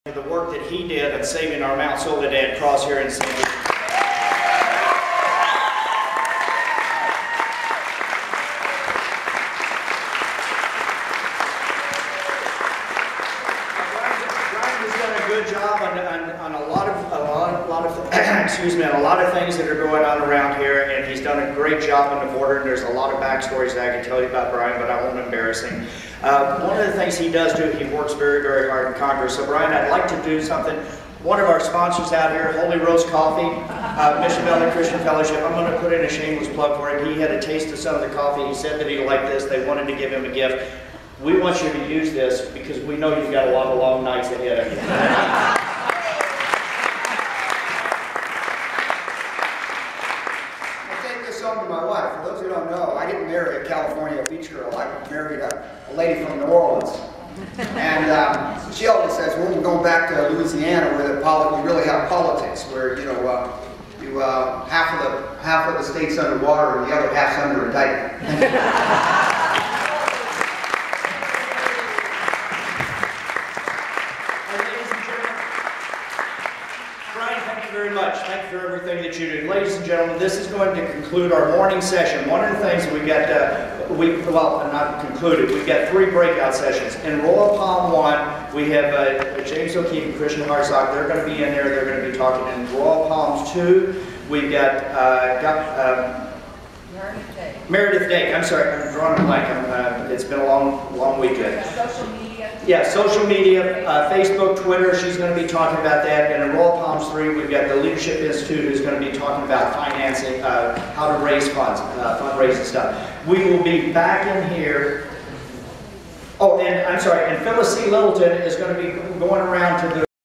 Brian Bilbray at Eagle Forum SD -3
Rep Brian Bilbray speaks at Eagle Forum San Diego convention, clip 3